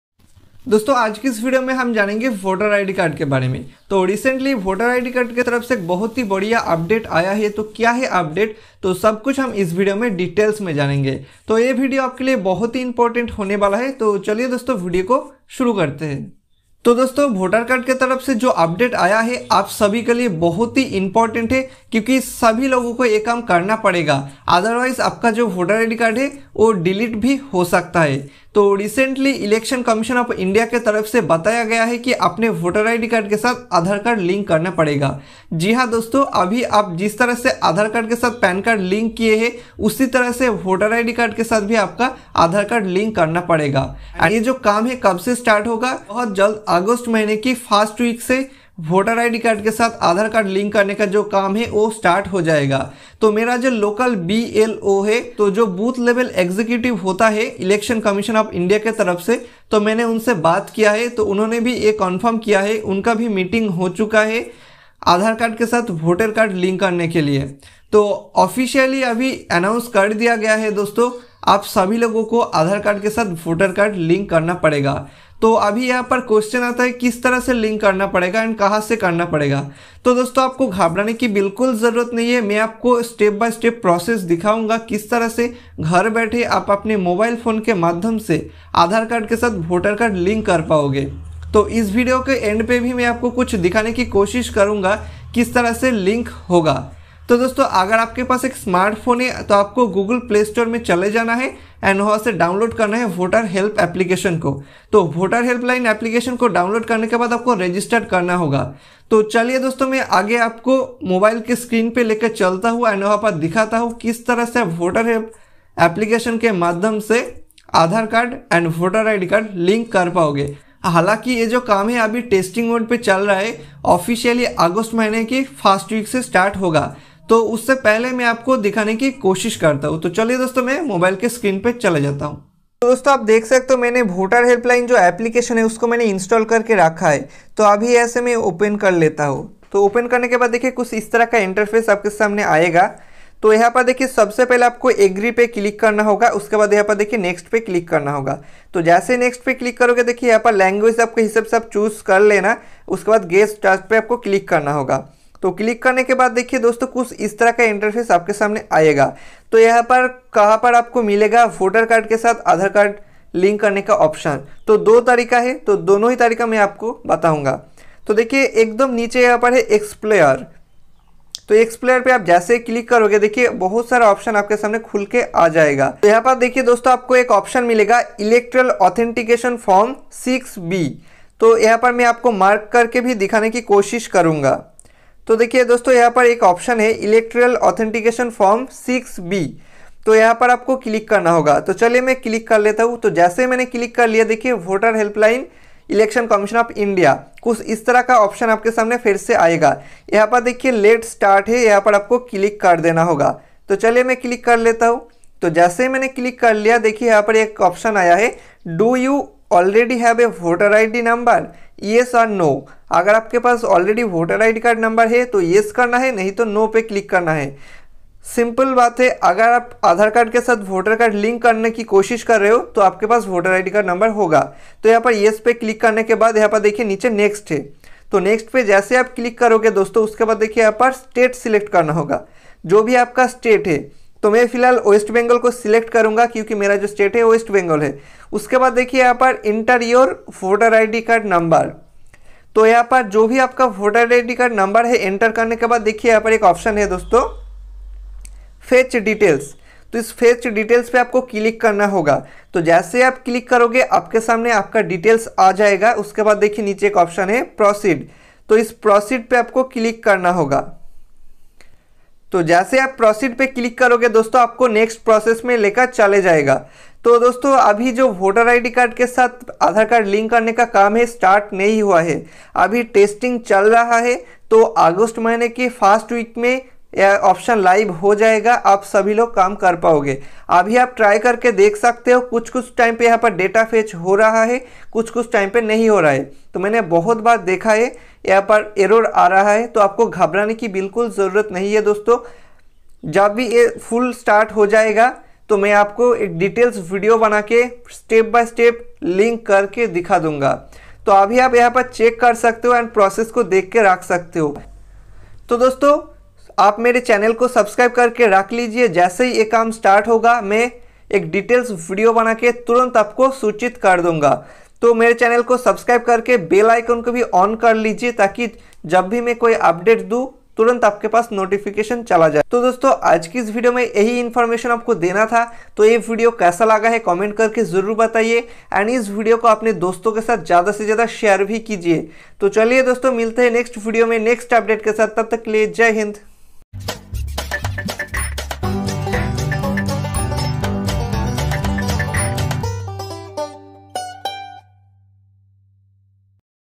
How to link Voter Card & Aadhaar Card | Voter Card Aadhar Card Link 2022
Dosto aj ke is video me humne bataya hai voter id card and aadhaar card link kese kare so a video apkaliye bhate important hai.

Thanking you
Technical Episode Pro